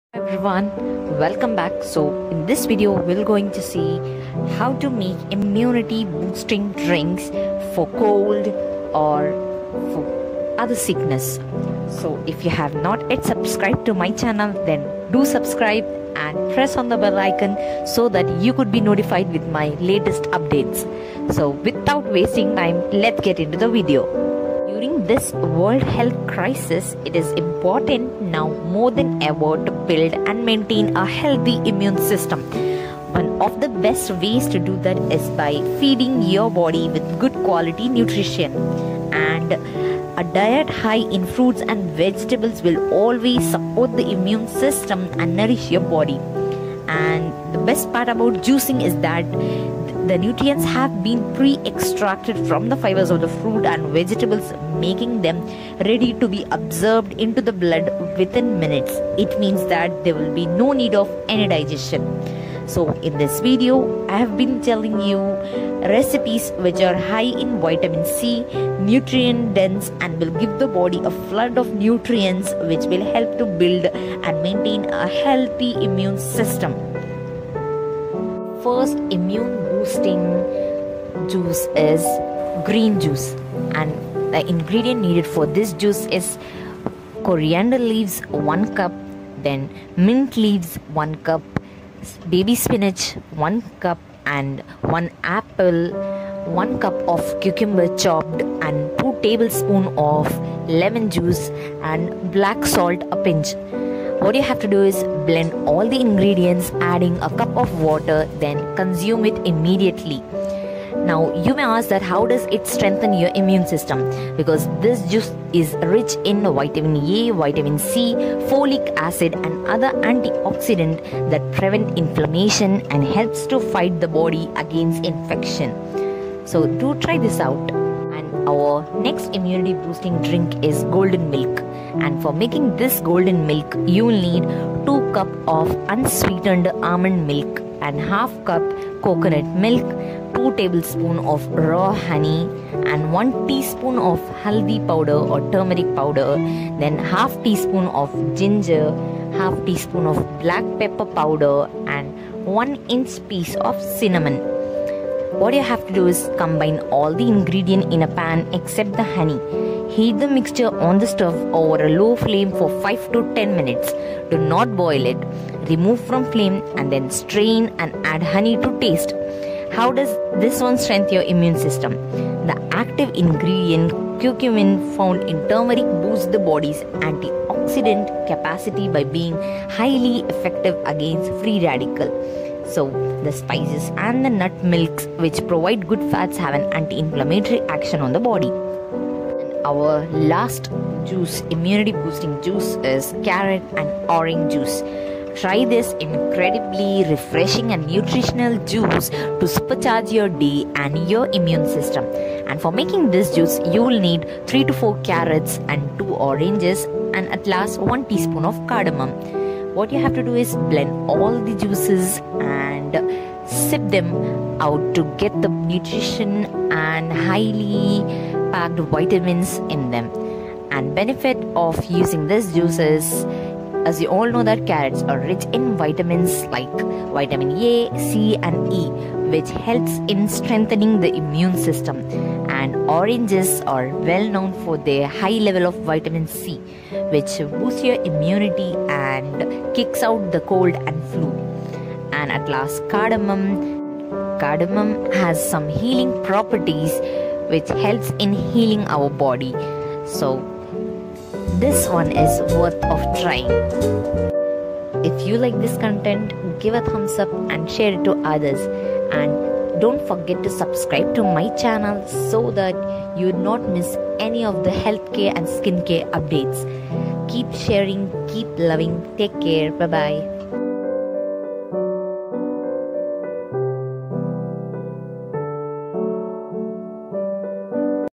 Immunity Boosting Drink | HEALTHY DRINK FOR IMMUNITY | IMMUNITY DRINK FOR CORONA VIRUS
IMMUNITY BOOSTING DRINK ,HOW TO MAKE IMMUNITY BOOSTER DRINK
 This drink is very healthy and increase our immunity system so that we can fight from any flu, cold, and Corona virus.

3 IMMUNITY DRINK FOR CORONA VIRUS

#1.Immune boosting green juice
Immune boosting green juice
Ingredients:
Baby spinach – 1 cup
Apple or pear – 1
Cucumber – 1 cup chopped
Lemon – 2 tbsp
Black salt – a pinch

Method:
Blend all ingredients adding a cup of water. Consume immediately.

How does it strengthen your immune system
This juice is rich in Vitamin A, Vitamin C, Folic acid and other antioxidants that prevent
inflammation and help the body fight infections.

#2 Golden milk
Golden Milk
Ingredients:
2 cups of unsweetened almond milk
½ cup light coconut milk
2 tbsp raw honey
1 tsp Haldi powder (turmeric) or fresh grated turmeric
½ tsp ginger, ground
½ tsp black pepper powder
1-inch piece of cinnamon

Method:
Combine all the ingredients in a pan except the honey.
Heat the mixture on the stove over a low flame for 5-10 minutes. Do not boil.
Remove from flame, strain and add honey to taste.

The active ingredient, curcumin, found in turmeric boosts the body’s antioxidant capacity by being highly effective against free radicals. The spices and the nut milks which provide good fats have an anti-inflammatory action on the body.

#3. Carrot. Orange. Cardamom.
Try this incredibly refreshing and nutritional juice to supercharge your day and your immune system.

Ingredients:

3–4 carrots
2 oranges
1 teaspoon cardamom
Benefits:

Carrot: Carrots are full of beta carotene, which strengthens the body and protects against the free radical damage that can lead to illness. Carrots are also chock full of many important vitamins (such as vitamins A, C, and E), carotenoids, and polyphenols that offer a variety of health benefits, including strengthening the immune system.
Orange: Many people reach for orange juice when trying to fight off the common cold. Oranges are well-known for their high level of vitamin C, which can boost your immunity and be an asset during the cold and flu season. Vitamin C has been studied extensively and may help decrease the duration and intensity of the common cold.
Cardamon: Cardamom is a warming spice often found in eggnog, spicy chai, and garam masala. While its taste is surely a winner for many dishes and libations worldwide, cardamom also has some healing properties worth noting. Cardamom contains an active compound called eucalyptol. Although further research is needed, one study has found that eucalyptol has anti-inflammatory and immunomodulatory potential. Cardamom also has antimicrobial and antioxidant properties, and research has found that cardamom contains compounds that can help prevent or suppress the proliferation of cancer cells. In addition to all of these health benefits, cardamom adds a warming, tasty flavor and some zip to your juice.

watch this video fully